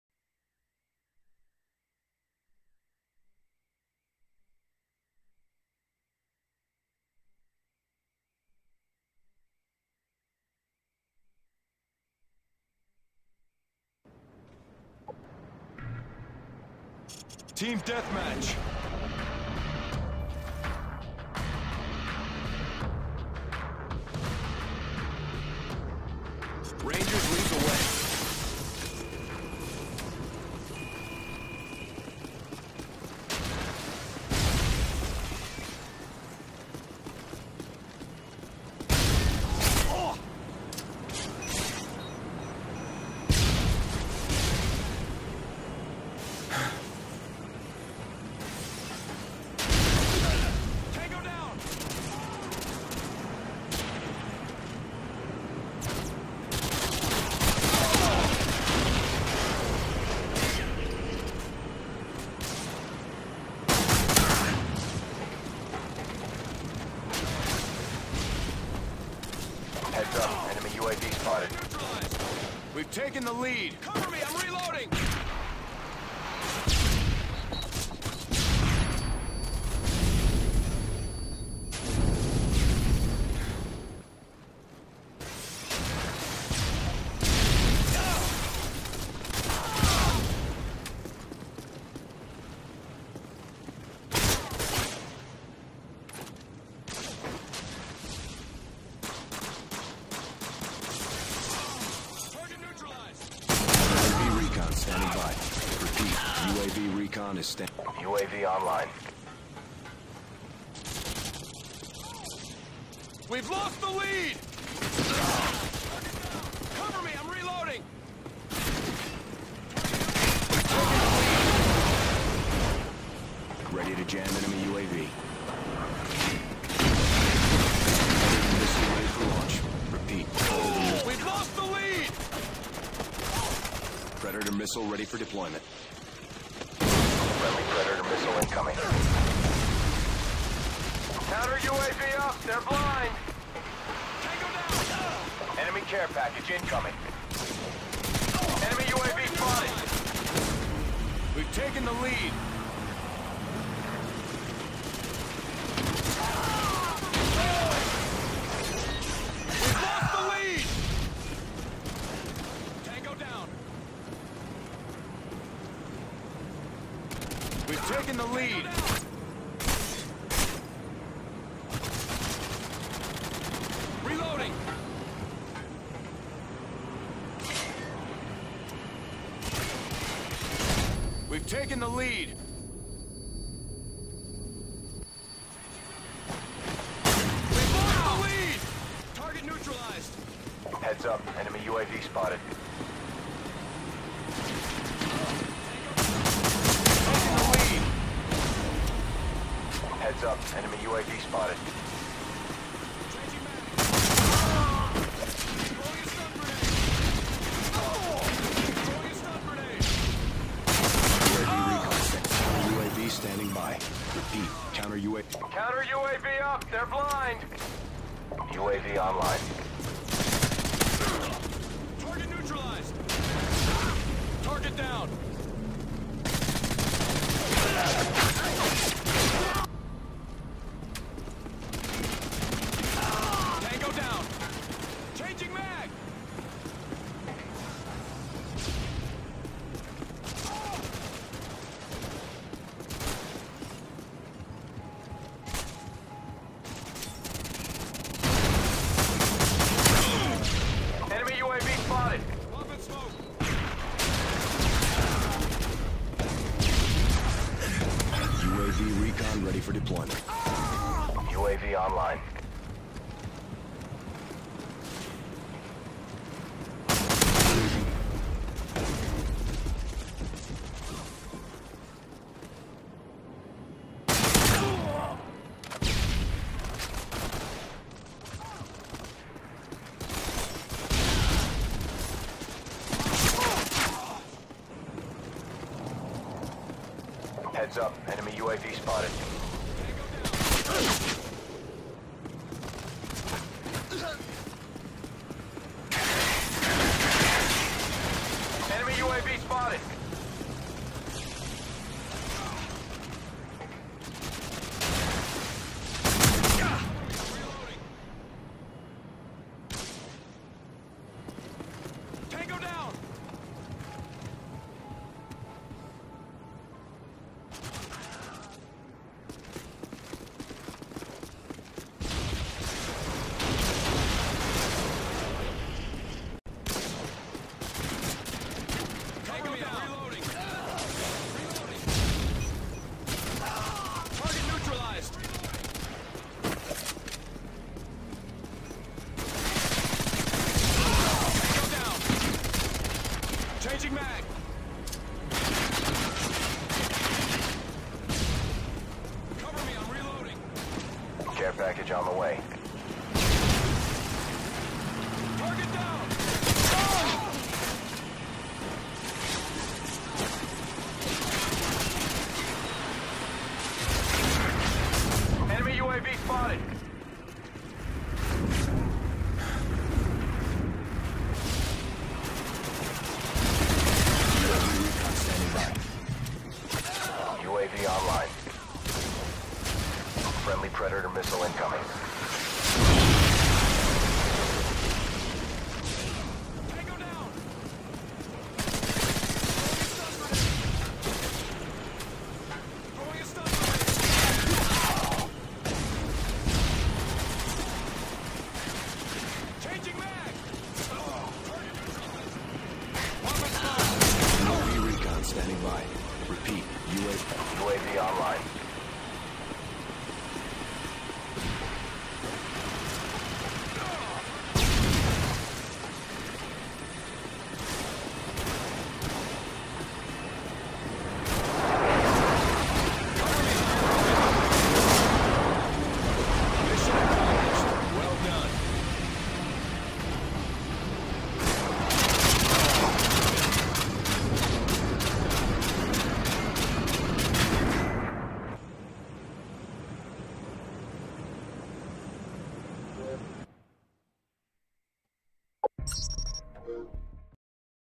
TDM 25-2-9 :: Terminal
Just playing some TDM on Terminal, go pretty well that game.